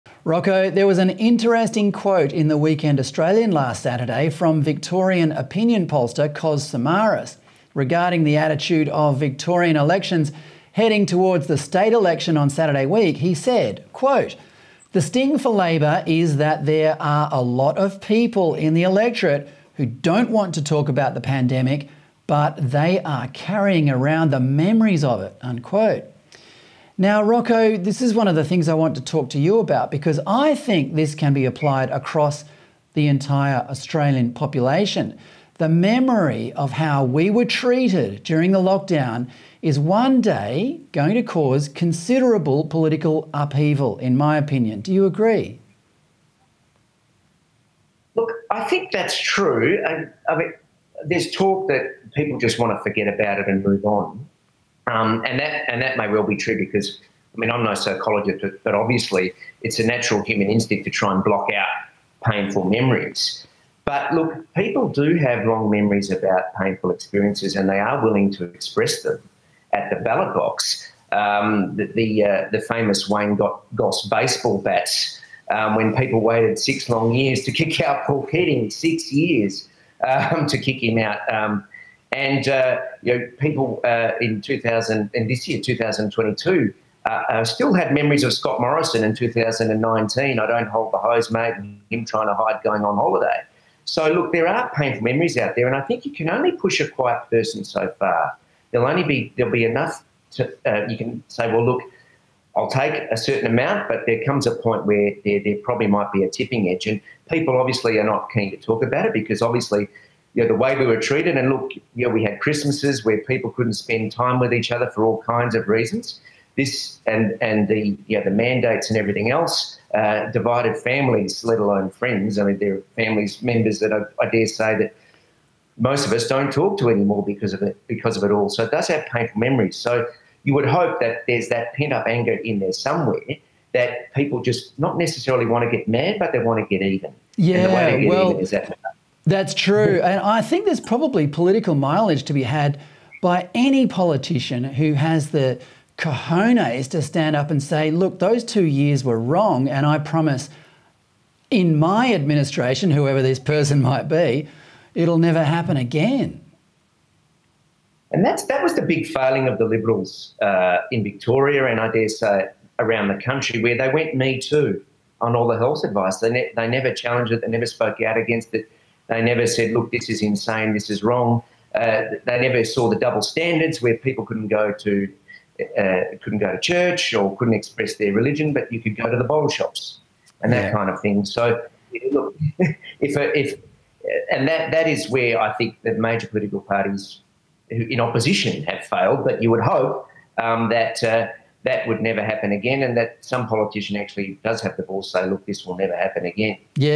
Australia must have an honest discussion about lockdowns: Rocco Loiacono | Fred Pawle
‘People are obviously not keen to talk about it because of the way we were treated. We had Christmases where people couldn’t spend time with each other for all kinds of reasons

Legal academic Dr. Rocco Loiacono joins Fred Pawle.